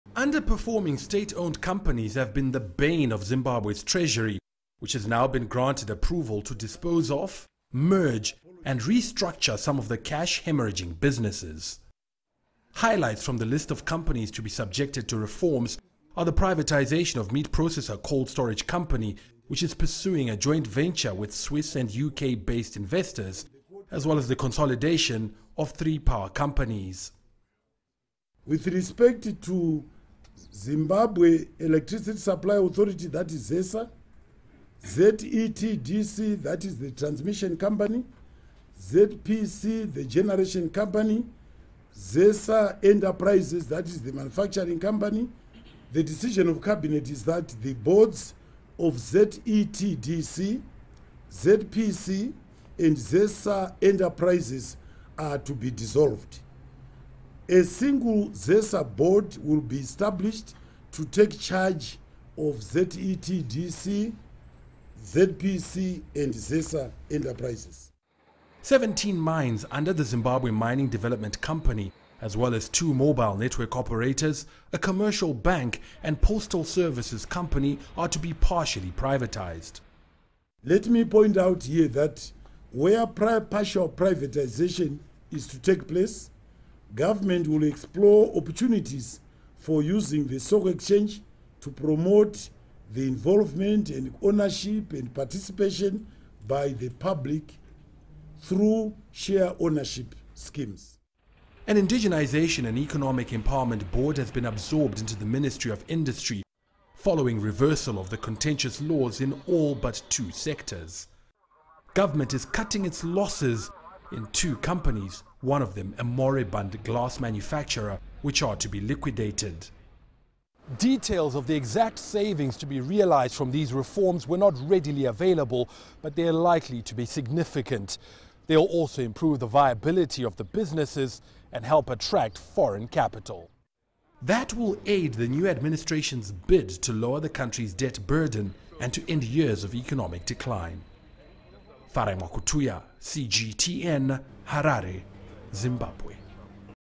Zimbabwe to sell, merge or restructure state enterprises
Zimbabwe plans to sell, merge or restructure more than 45 state enterprises to lower government expenditure and improve efficiency. President Emmerson Mnangagwa has made economic revival his top priority. CGTN's Farai Mwakutuya reports.